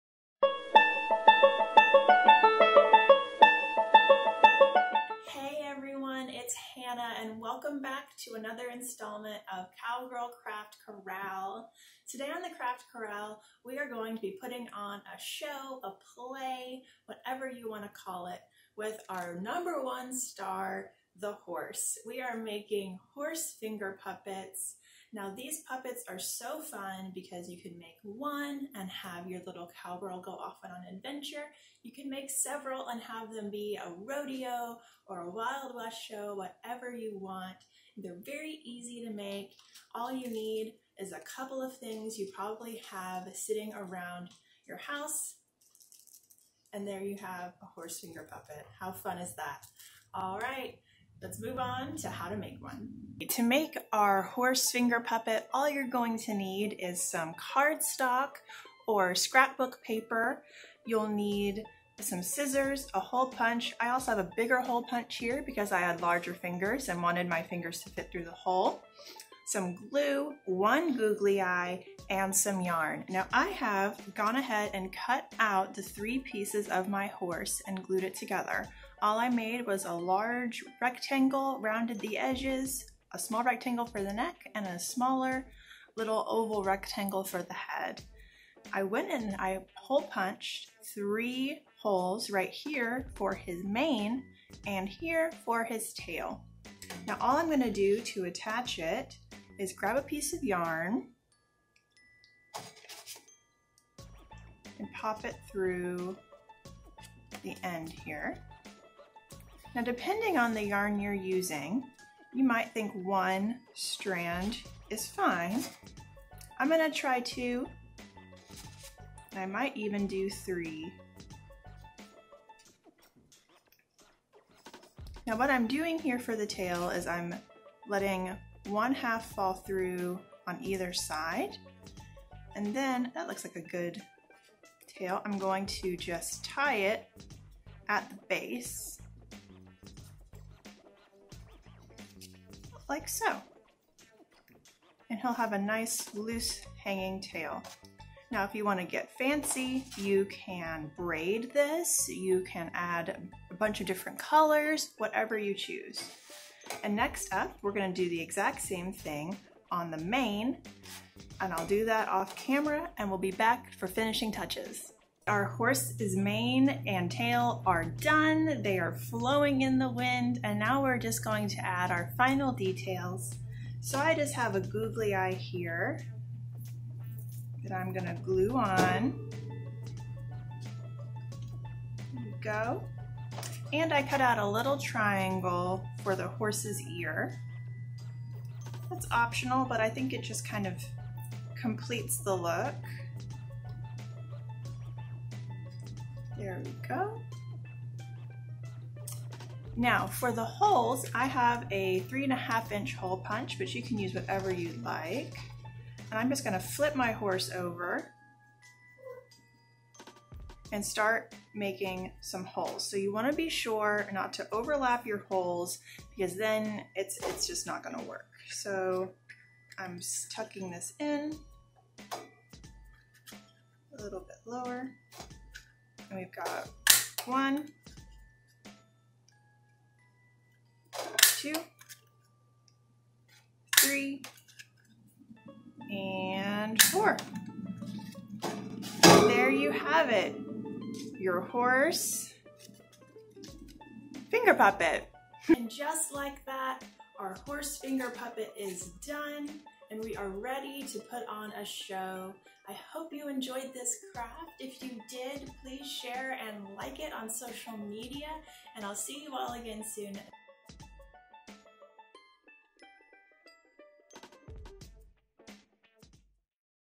Cowgirl Craft Corral Craft Horse Puppet
In this Cowgirl Craft Corral we are making a horse puppet with quick and easy steps to follow. Then you can host your own horse show, rodeo, or trail riding adventures! Join in with materials you most likely have at home.